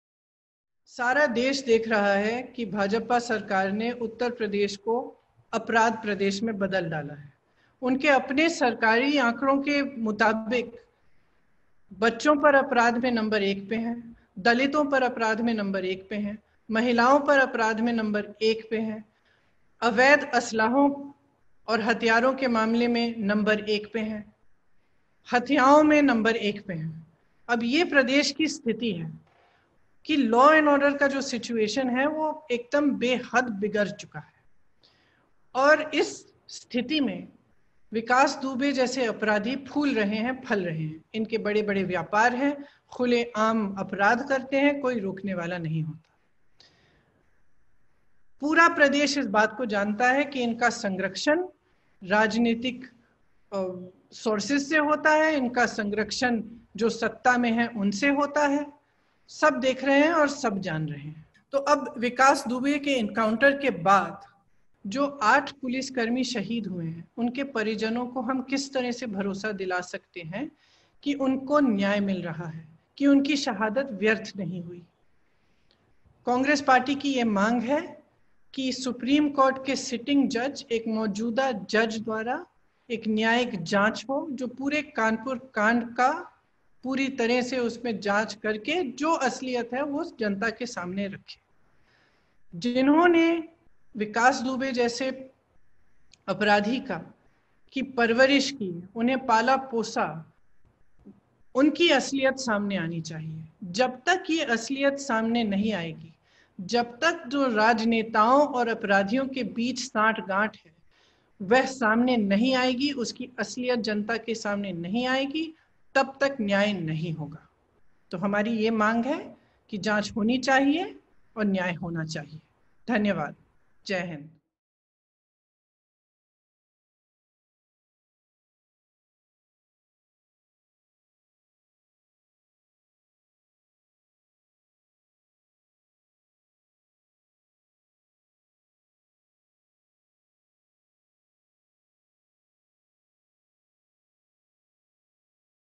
Priyanka Gandhi ने Vikas Dubey Encounter पर उठाए सवाल, कहा सुप्रीम कोर्ट के जज करें जांच - News360Tv
विकास दुबे के एनकांउटर पर प्रियंका गांधी ने योगी आदित्यनाथ से सवाल पूछा है.. उप्र की कानून-व्यवस्था बदतर हो चुकी है। राजनेता-अपराधी गठजोड़ प्रदेश पर हावी है। कानपुर कांड में इस गठजोड़ की सांठगांठ खुलकर सामने आई।
प्रियंका गांधी ने एक वीडियो के जरिए ट्वीट कर कहा है कि उप्र की कानून-व्यवस्था बदतर हो चुकी है। राजनेता-अपराधी गठजोड़ प्रदेश पर हावी है। कानपुर कांड में इस गठजोड़ की सांठगांठ खुलकर सामने आई। कौन-कौन लोग इस तरह के अपराधी की परवरिश में शामिल हैं- ये सच सामने आना चाहिए। सुप्रीम कोर्ट के मौजूदा जज से पूरे कांड की न्यायिक जाँच होनी चाहिए